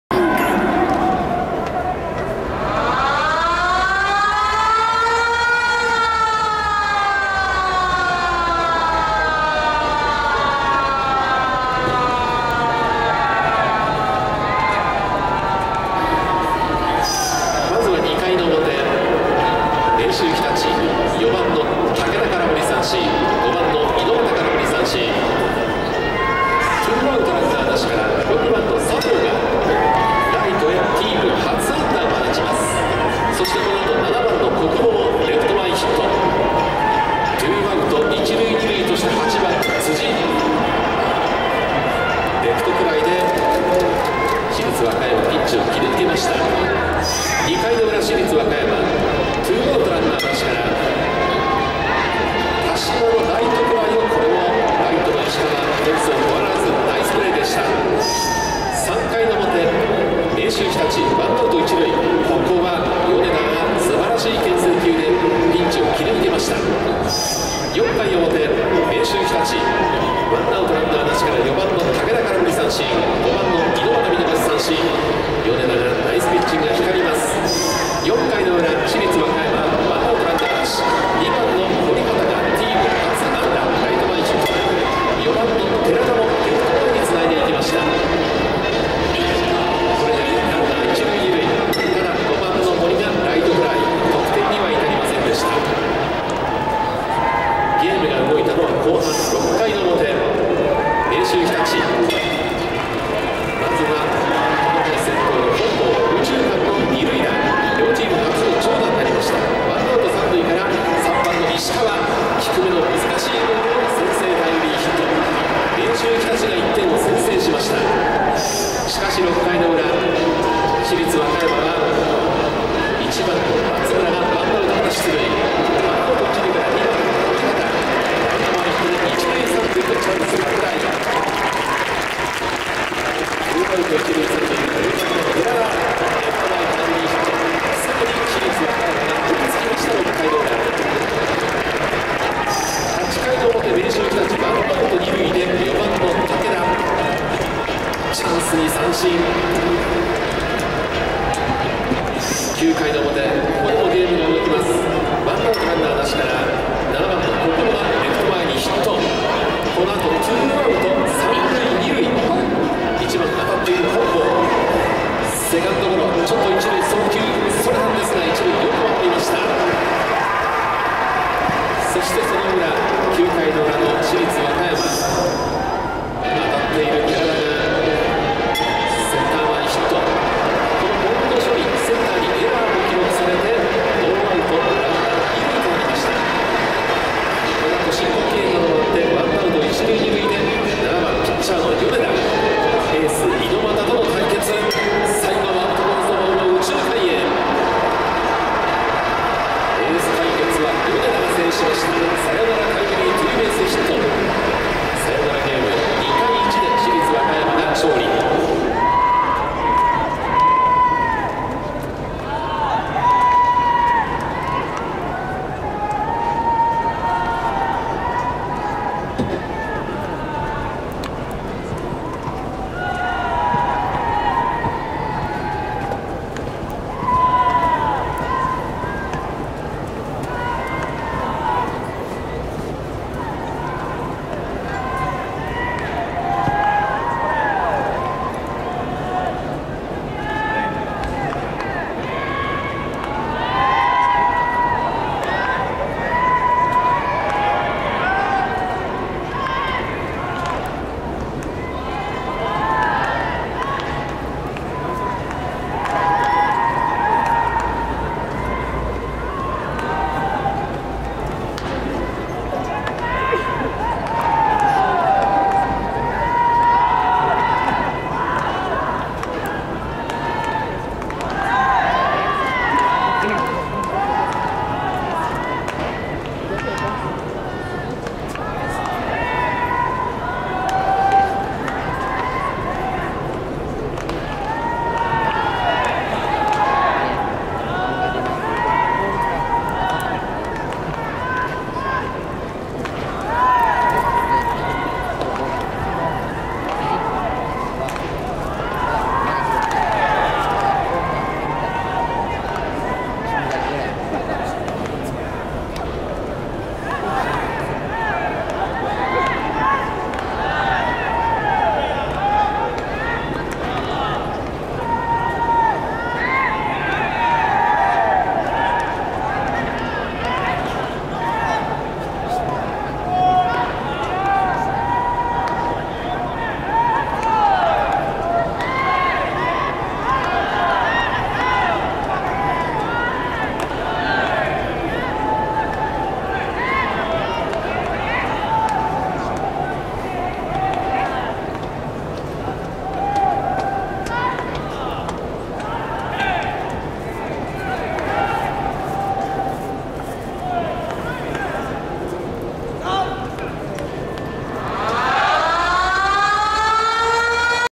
市立和歌山 シートノック （2022年3月28日 センバツ準々決勝）
市立和歌山　スタメン
1（捕） 松村祥吾　 3年生 　紀州由良リトルシニア
2（二） 堀畑樹 　　3年生 　 貝塚リトルシニア
3（一） 大池悠太　 3年生 　和歌山田辺ボーイズ
4（中）寺田椋太郎 3年生 　和歌山打田タイガース
5（左） 森大輔 　　3年生 　中百舌鳥ボーイズ
6（三） 田嶋優汰　 2年生　 和歌山田辺ボーイズ
7（右） 山崎僚也 　3年生 　紀州ボーイズ
8（投） 淵本彬仁 　3年生　 和歌山ビクトリーズ
9（遊） 熊本和真 　2年生　 和歌山リトルシニア